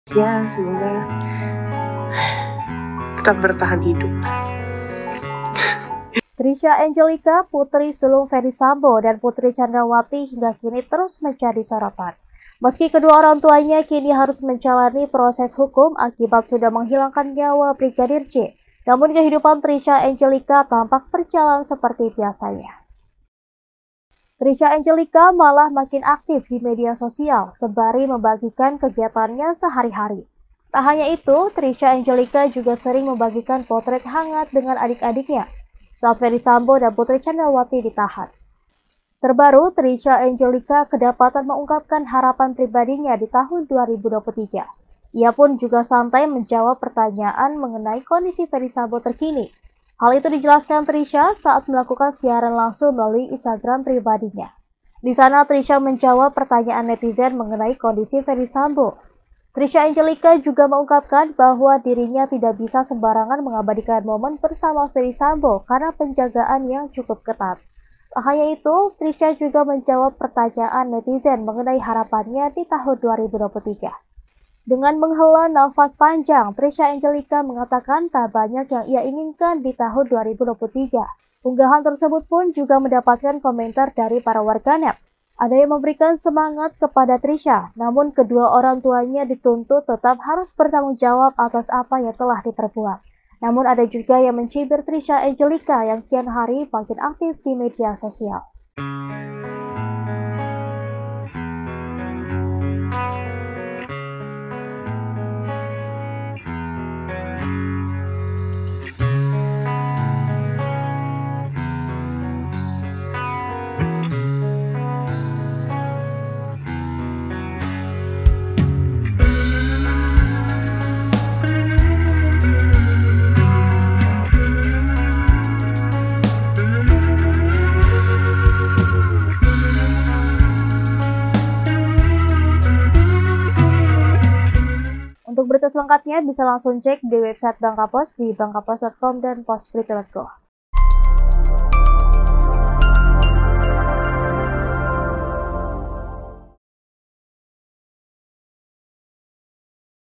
Ditanya Soal Harapannya di 2023, Anak Sulung Sambo Ingin Satu Hal Ini
Trisha Eungelica putri sulung Ferdy Sambo dan Putri Chandrawati hingga kini terus menjadi sorotan.

Meskipun kedua orang tuanya kini harus menjalani proses hukum akibat sudah menghilangkan nyawa Brigadir J, namun kehidupan Trisha Eungelica tampak berjalan seperti biasanya.